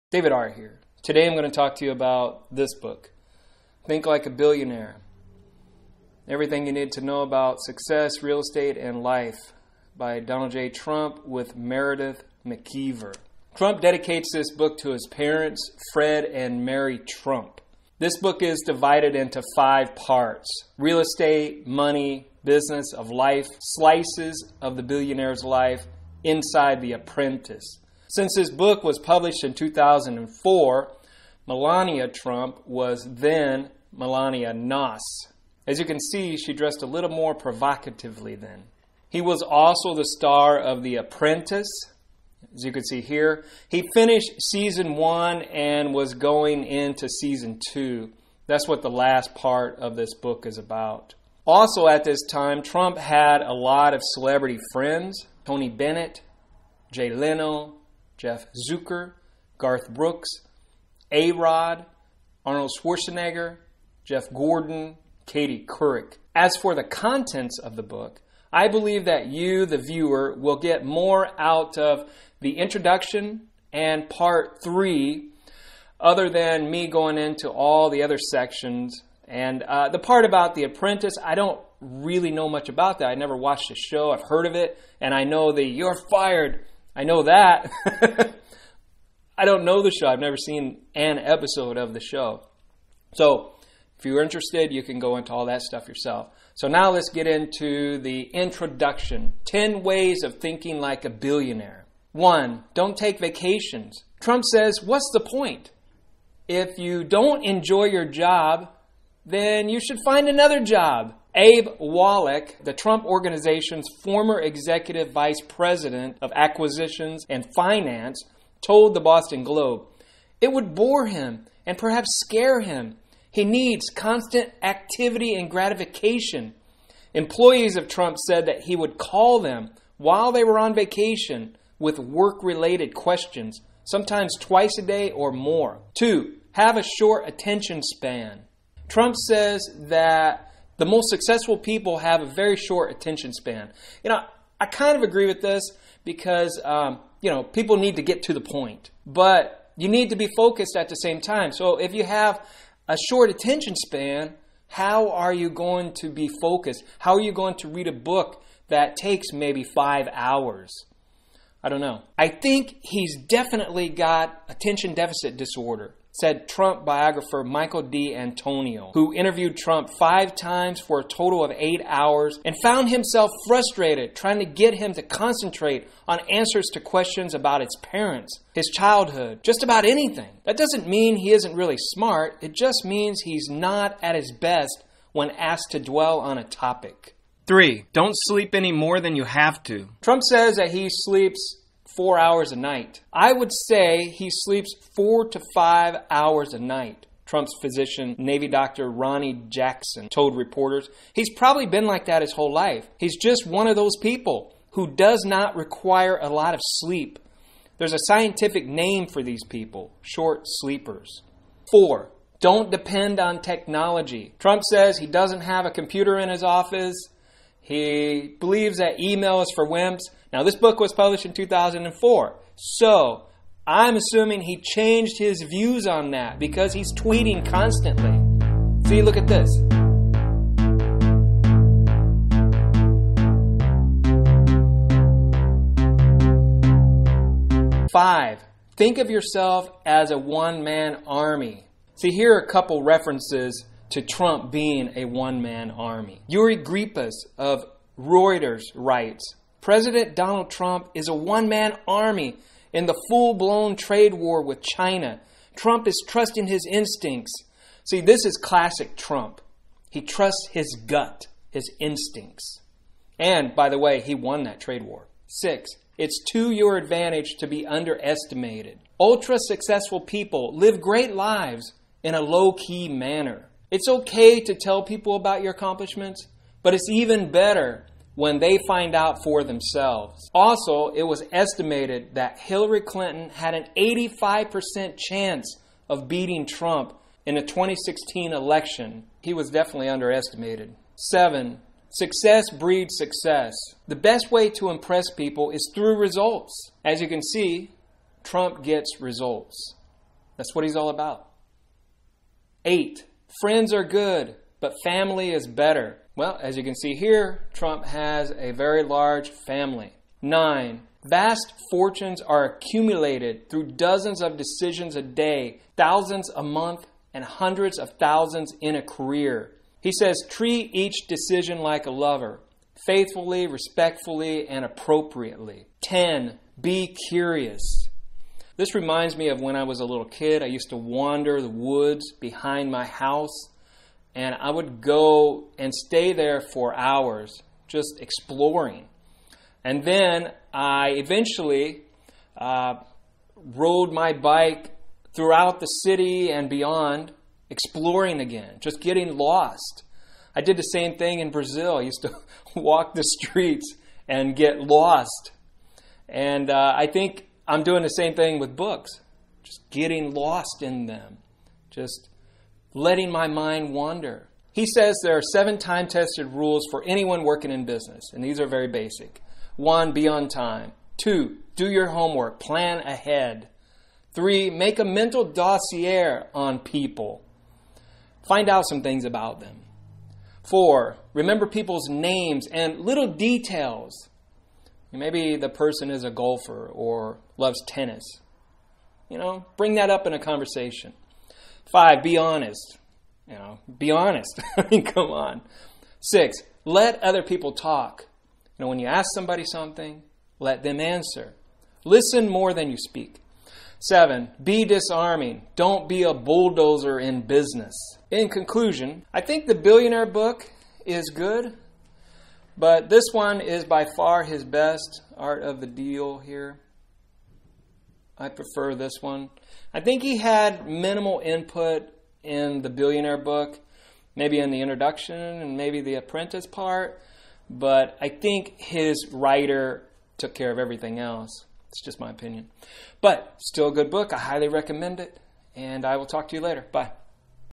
TRUMP: THINK LIKE A BILLIONAIRE by DONALD J. TRUMP with MEREDITH McIVER
(AMAZON.COM DESCRIPTION)

It’s not good enough to want it. You’ve got to know how to get it. Real estate titan, bestselling author, and TV star Donald J. Trump is the man to teach you the billionaire mind-set–how to think about money, career skills, and life. Here is crucial advice on investing in real estate from the expert, everything from dealing with brokers to renovating to assessing the value of property, buying and selling, and securing a mortgage. Trump will show you how to cut costs, decide how much risk to assume in your investments, and divide up your portfolio. He’ll also teach you how to impress anyone, how to correct or criticize someone effectively, and how to know if your friends are loyal–everything you need to know to get ahead.

And once you’ve earned your money, you’ve got to learn to spend it well. Trump presents his consumer guide to the best things in life, from wine to golf clubs to engagement rings. Check out the billionaire lifestyle–how they shop and what they buy. Even if you’re not superwealthy, you can afford many of these luxuries.

And what look inside the Trump world would be complete without The Apprentice? Trump will take you behind the scenes, from the end of season one and into season two, with insights into the making and the meaning of TV’s hottest show.
As Donald Trump proves, getting rich is easy. Staying rich is harder. Your chances are better, and you’ll have more fun, if you think like a billionaire. This is the book that will help you make a real difference in your life.